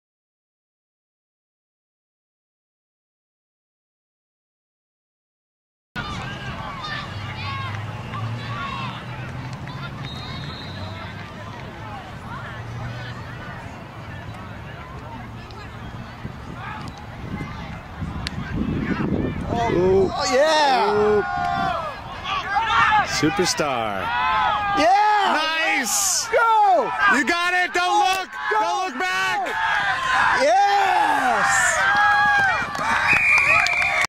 9/18/2010 - PAC Rugby Highlight of the Week vs Baltimore-Chesapeake
PAC rugby scores in Charm City.  Irak Herazo steps and straight-arms a last-chance tackler.